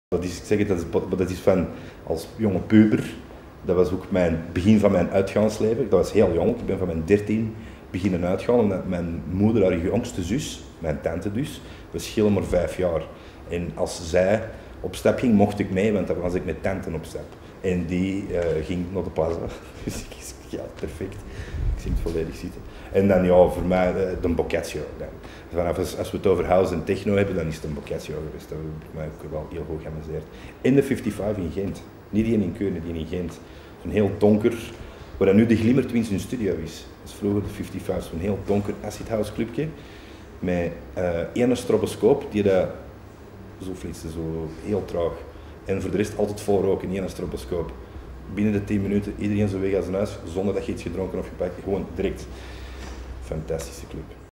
CJ Bolland on his favourite clubs: Plaza, Boccaccio & 55 Ghent
Interview recorded July 17 2009 at Café Capital, Antwerp. Copyright of